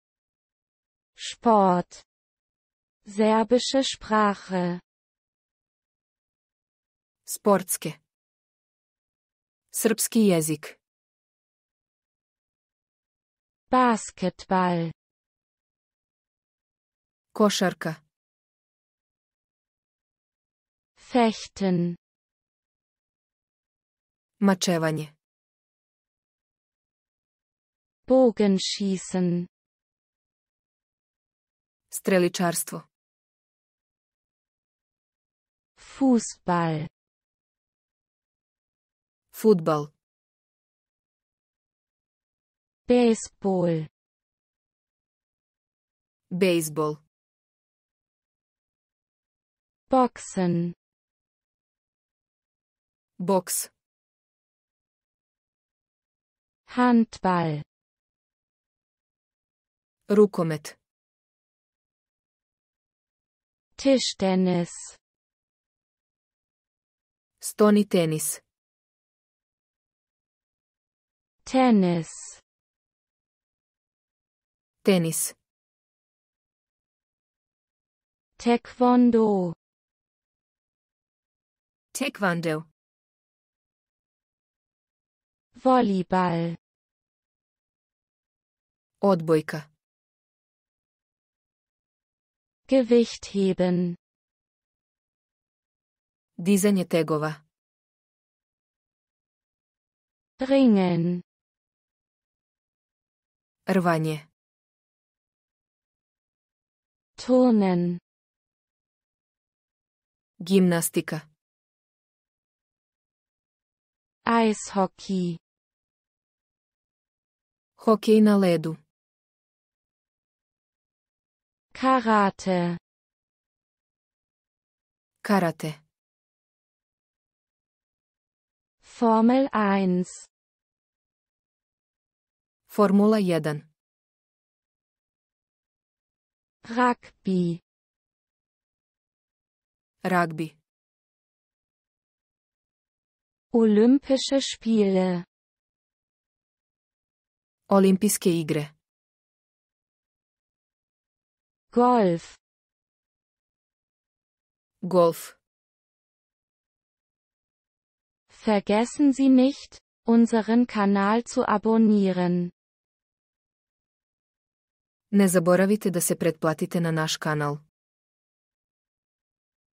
Sport (Serbische Sprache) (de-sr)
Fone TV
de-sr-24
спортске (Српски језик)
Basketball - Кошарка
Fechten - Мачевање
Bogenschießen - Стреличарство
Fußball - Фудбал
Baseball - бејзбол
Boxen - Бокс
Handball - Рукомет
Tischtennis - Стони тенис
Tennis - Тенис
Taekwondo - Tekvondo
Volleyball - Одбојка
Gewichtheben - Дизање тегова
Ringen - Рвање
Turnen - гимнастика
Eishockey - Хокеј на леду
Karate - Карате
Formel 1 - Формула 1
Rugby - Рагби
Olympische Spiele - Олимпијске игре
Golf - Голф